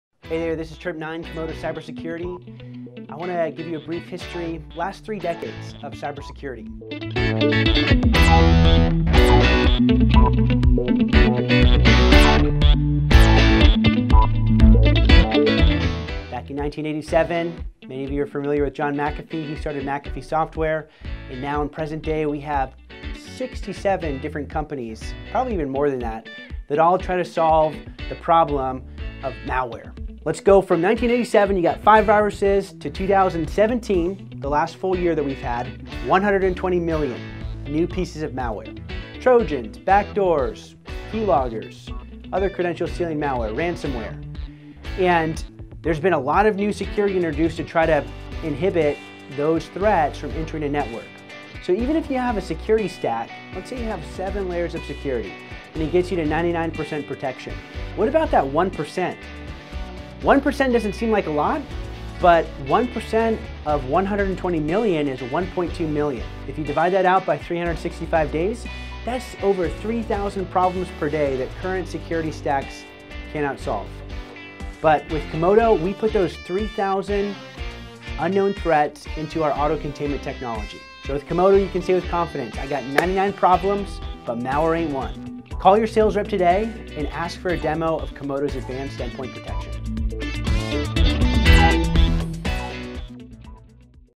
A Brief History of Cybersecurity
Only 5 viruses existed in 1987 to present day where over 120 million pieces of Malware exist and hundreds of companies are trying to find a solution for everything from Tojans, backdoors, to key loggers and other ransomware.

Learn more about Comodo Cybersecurity's unmatched Advanced Endpoint Protection and Auto containment technology